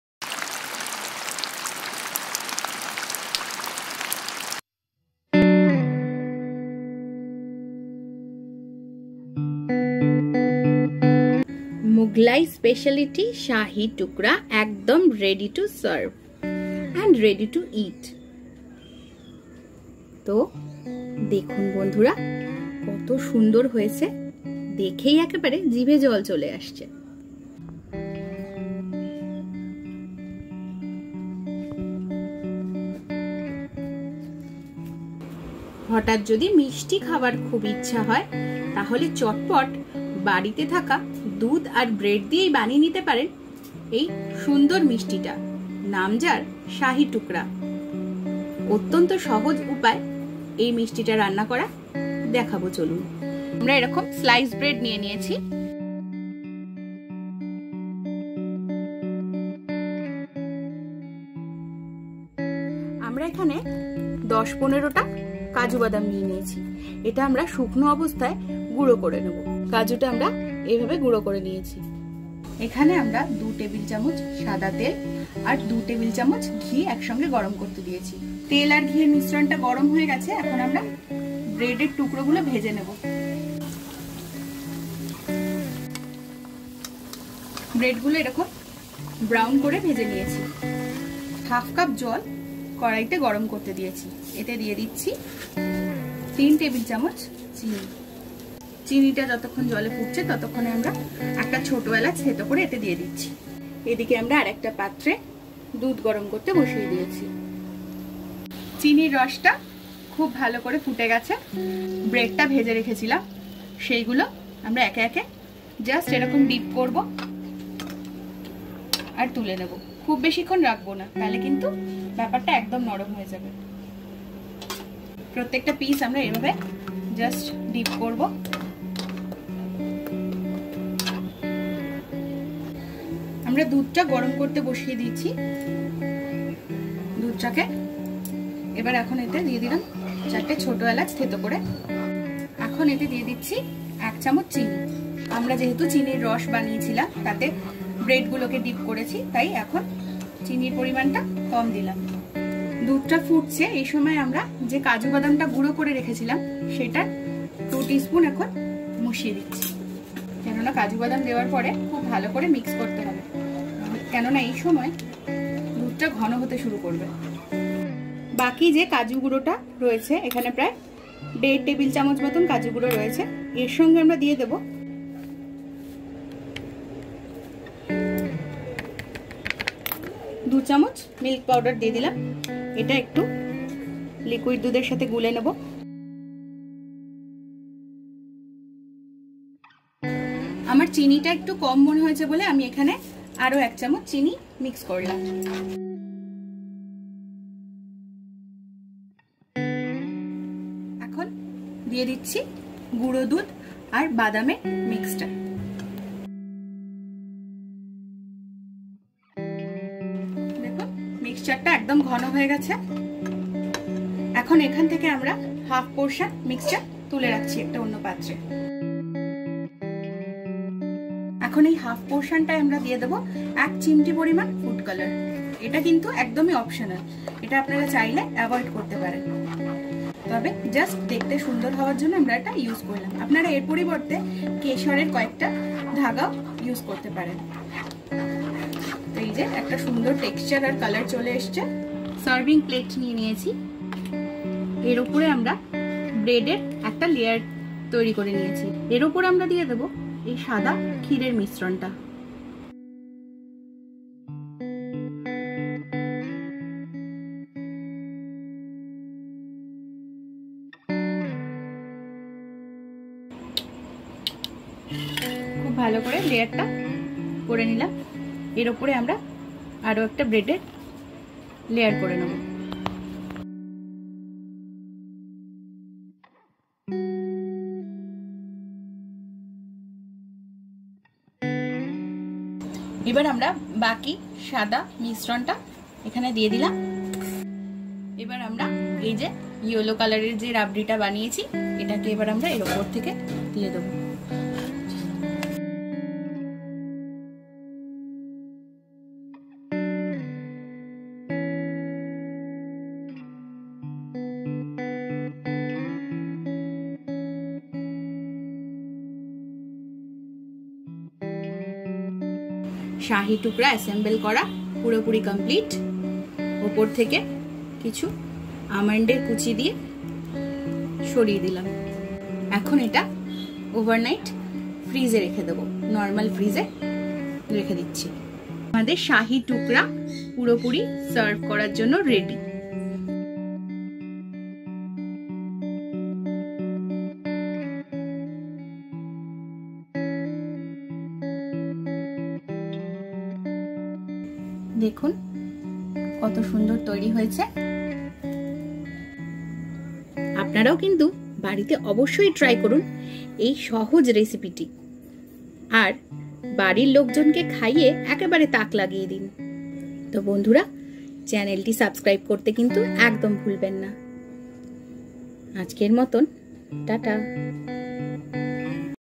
SHAHI TUKDA / SWEET RECIPE
Matro koek tukro bread r dudh diye toiri kora jabe ei recipe ti. khete osadharon.

This is a traditional MUGHLAI sweet recipe...
this is very mouthwatering recipe.